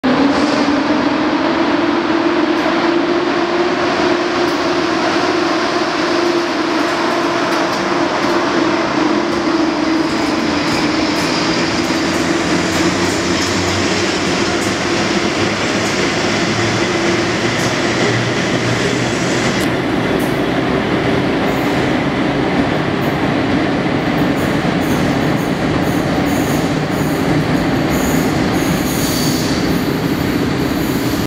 EP07-1017 + EU07-116 + EP09-019 + EP09-017 jako pociąg odjeżdżają ze stacji Warszawa Centralna.
Data nagrywania filmu: 27 czerwca 2013.